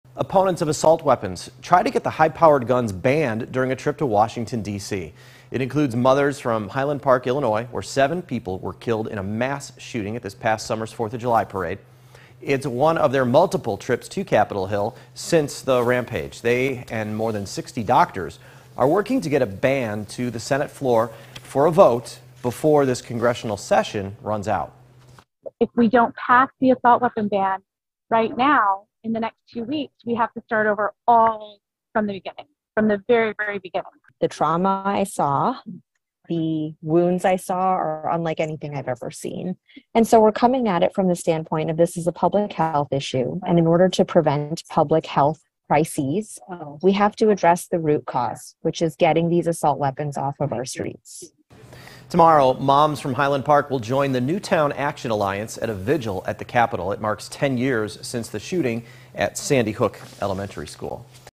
Illinois parents working to ban assault weapons
Opponents of assault weapons are trying to get the high-powered guns banned during a trip to Washington D.C.